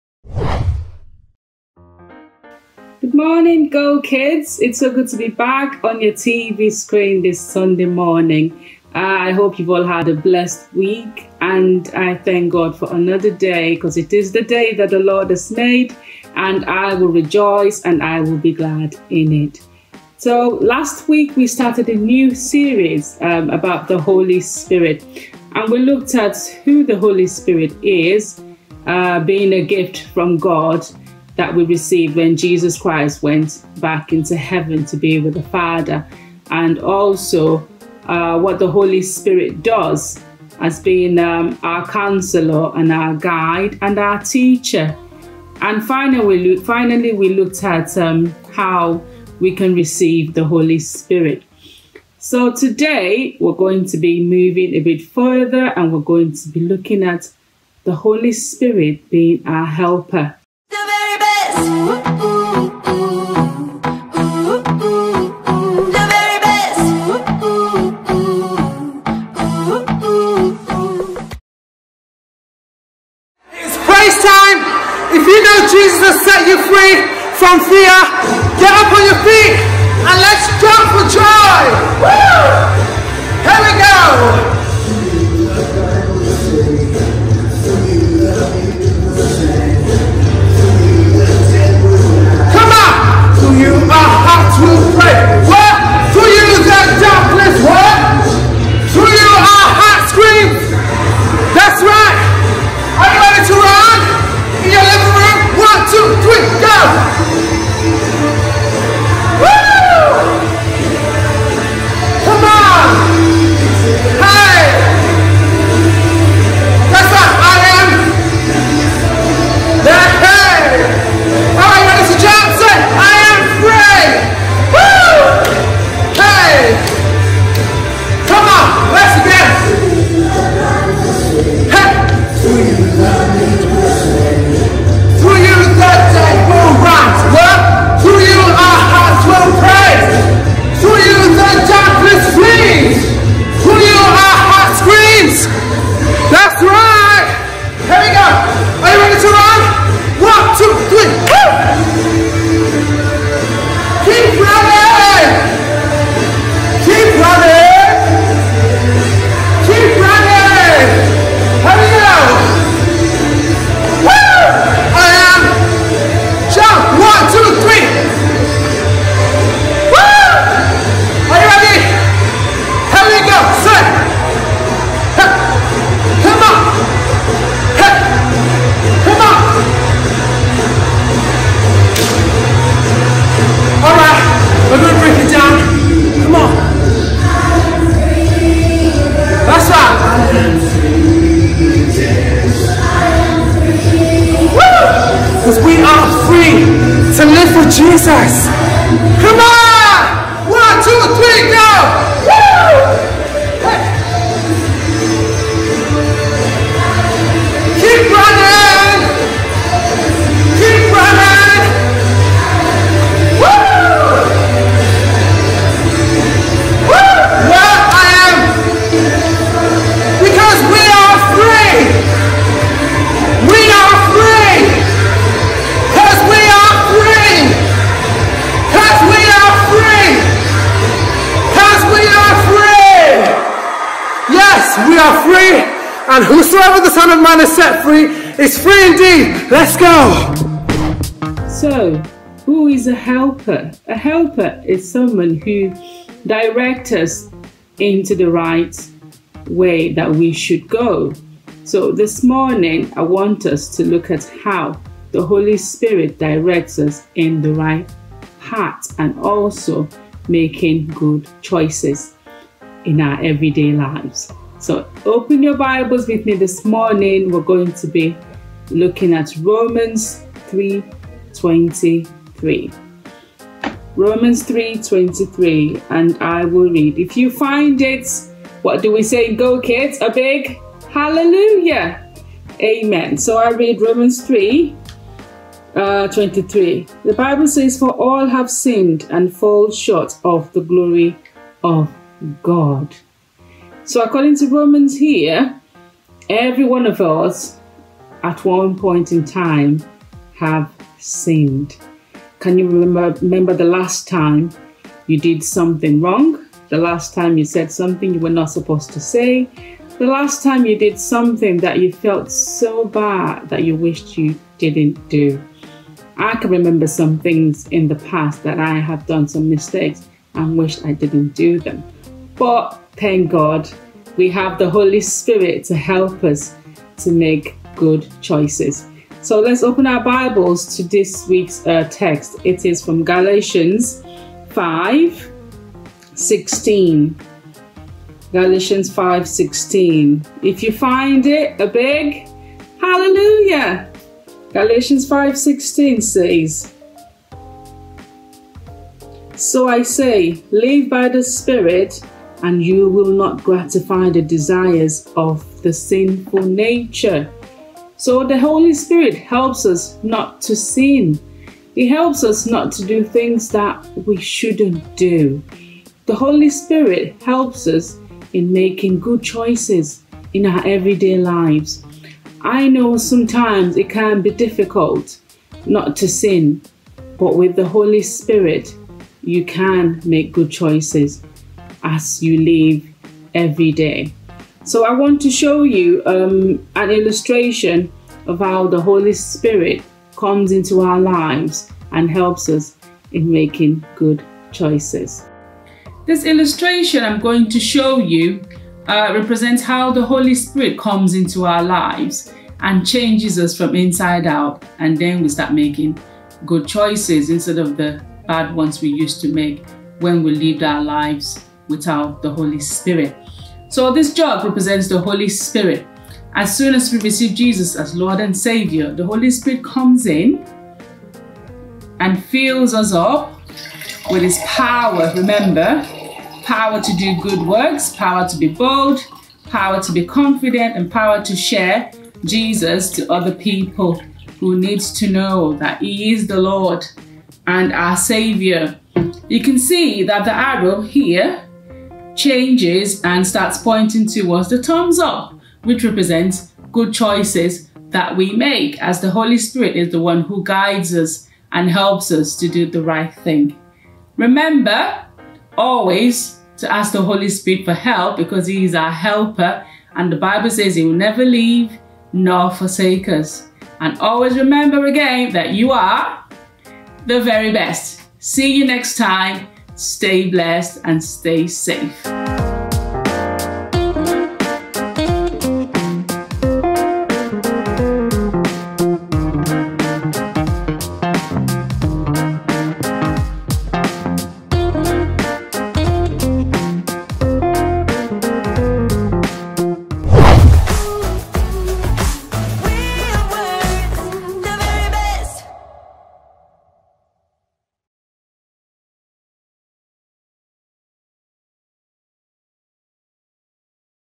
Help! | Sunday 20 September 2020 | GoKids
Join Joyce as we look at how the Holy Spirit helps us.